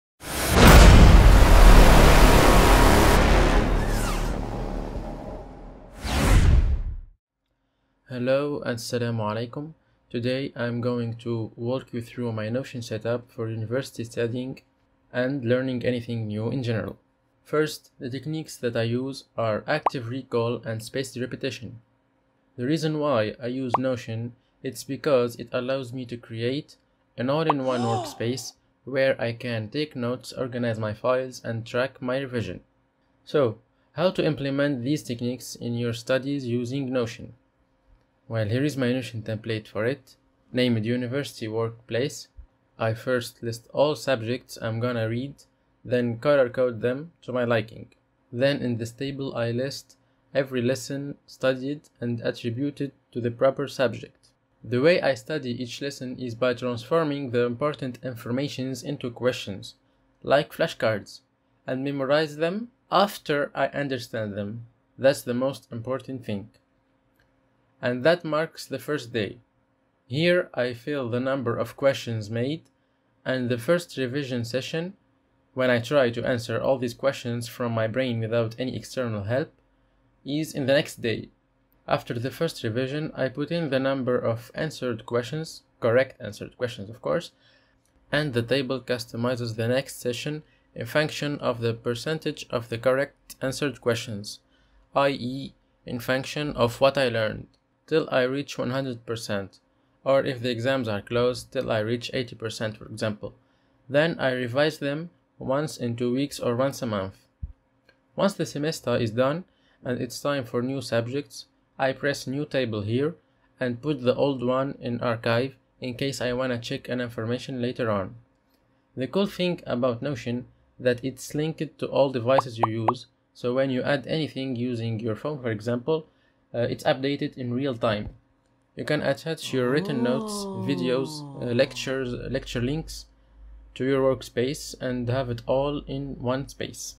How i Study With Notion + Free template (Active recall & Spaced Repetition)
in this short video i explain how i study using NOTION with a simple yet effective template (link below for free template) that implements these two techniques : spaced repetition & active recall i explained them
- the free template :